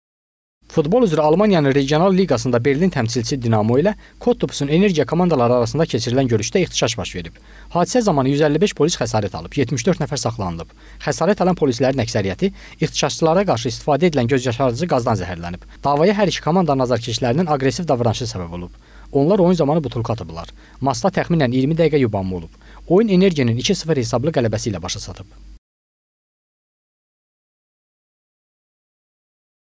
Futbol oyununda iğtişaş baş verdi, 155 polis xəsarət aldı – APA TV
Futbol üzrə Almaniyanın Regionla Liqasında Berlin təmsilçisi “Dinamo” ilə Kottbusun “Energie” komandaları arasında keçirilə görüşdə iğtişaş baş verib.Hadisə zamanı 155 polis xəsarət alıb, 74 nəfər saxlanılıb,
Xəsarət alan polislərin əksəriyyəti iğtişaşçılara qarşı istifadə edilən gözyaşardıcı qazdan zəhərlənib.Davaya hər iki komandanın azarkeşlərinin aqressiv davranışı səbəb olub.Onlar oyun zamanı butulka atıblar.
Matçda təxminən 20 dəqiqə yubanma olub.Oyun “Energie”nin 2:0 hesablı qələbəsi ilə başa çatıb.